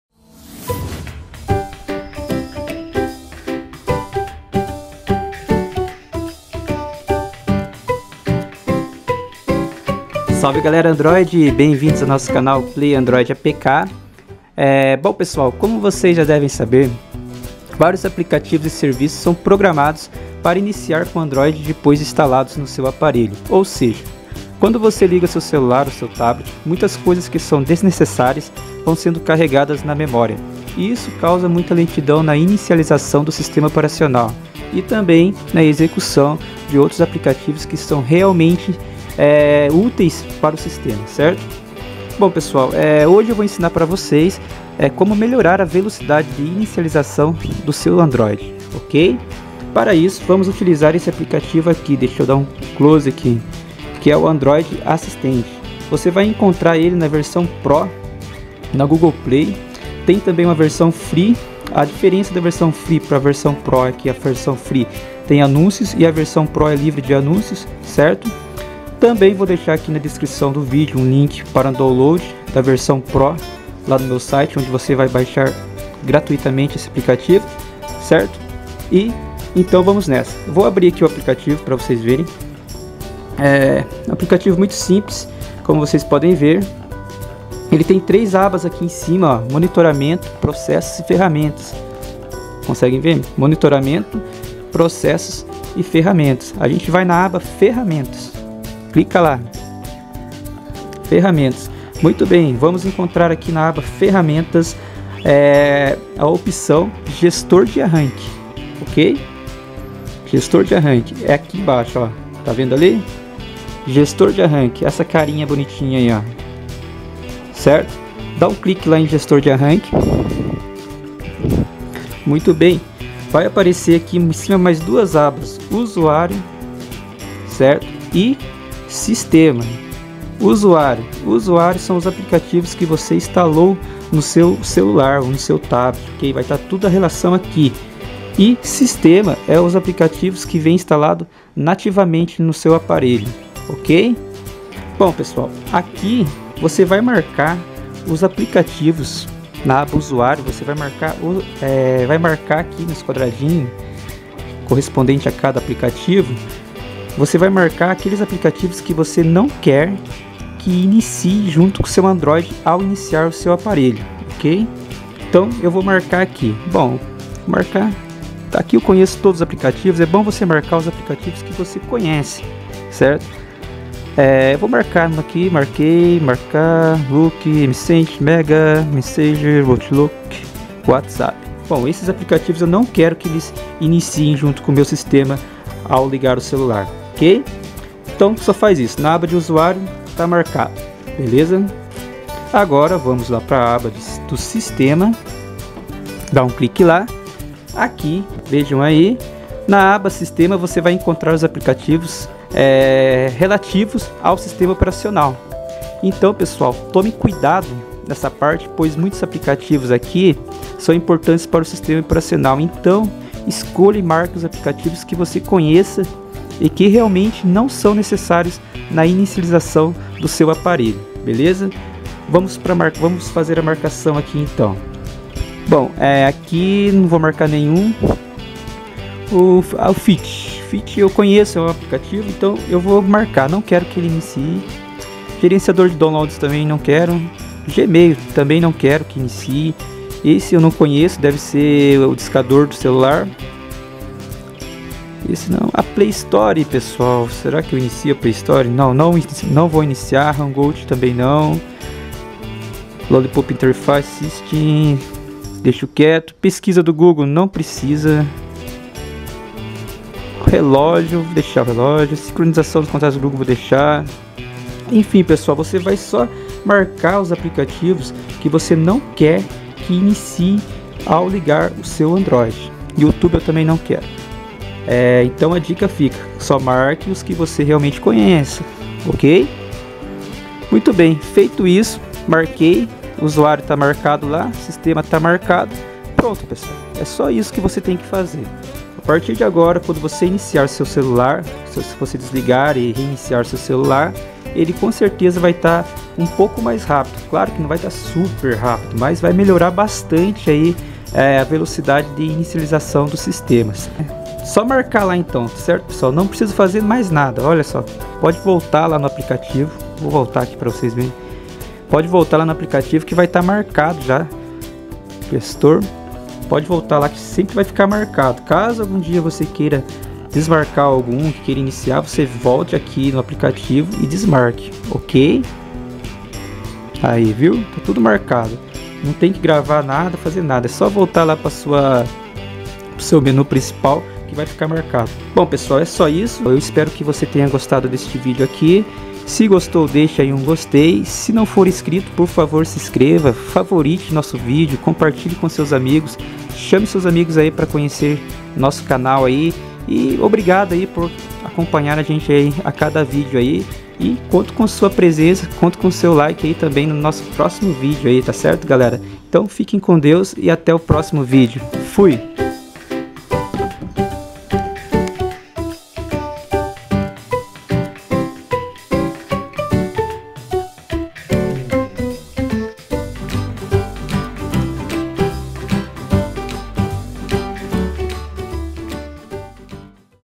Como melhorar a velocidade de inicialização do sistema android ao ligar o dispositivo
Obrigado por estar aqui e até o próximo vídeo! .